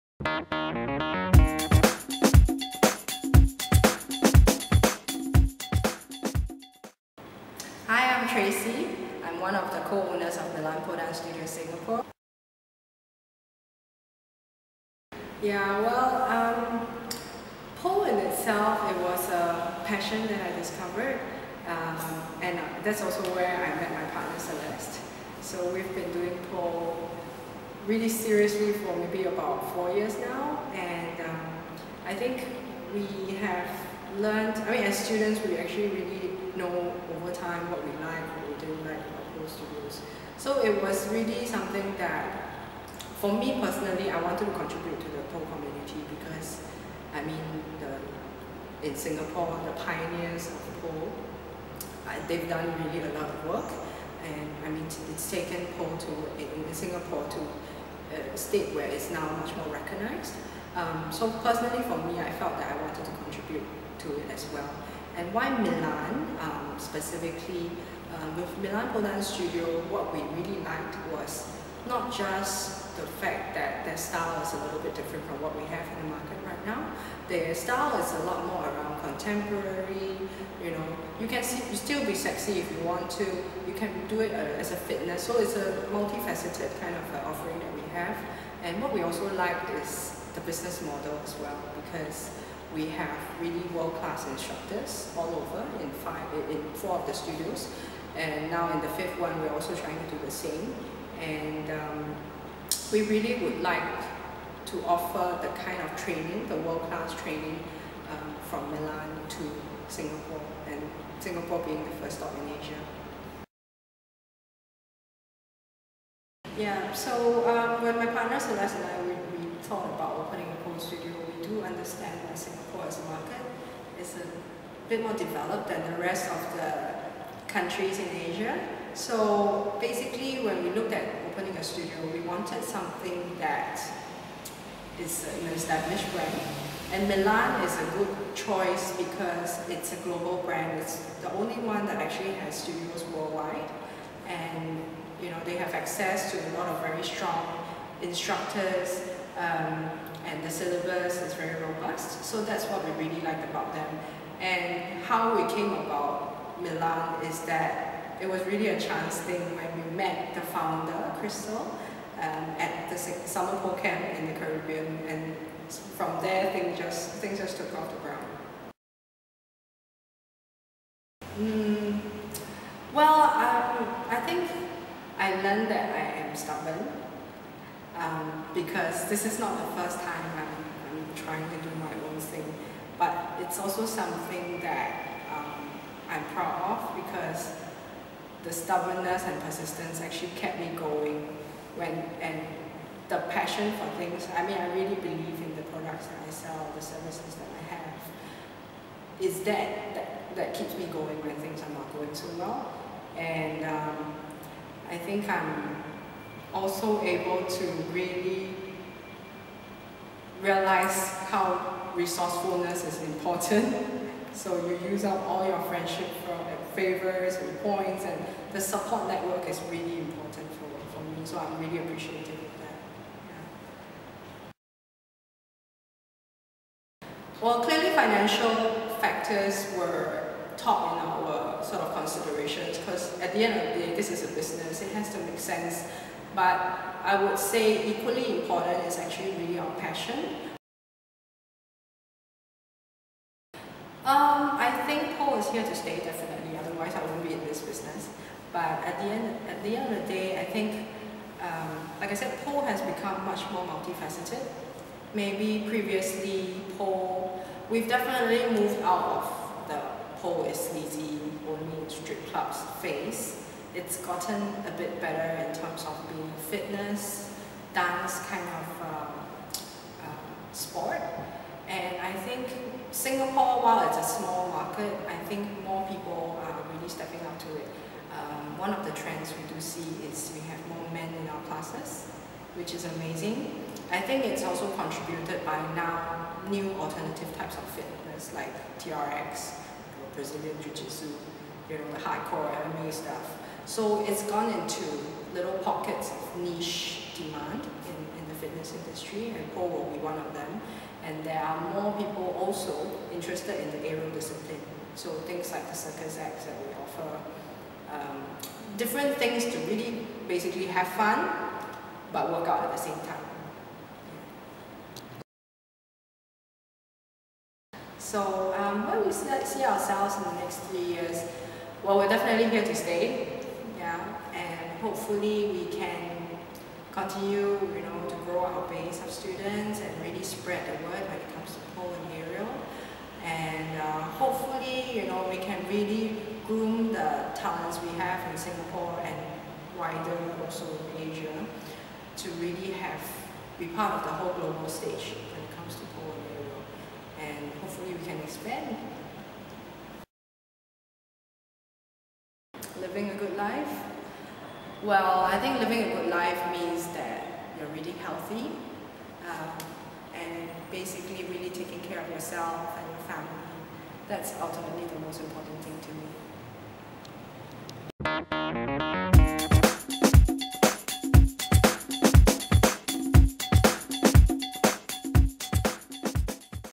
Milan Pole Dance Singapore Tracy Mak
International dance school, Milan Pole Dance Studio opened its first pole and aerial fitness studio in Asia, right here in Singapore in August 2014. We speak with local co-owner Tracy Mak and found out more.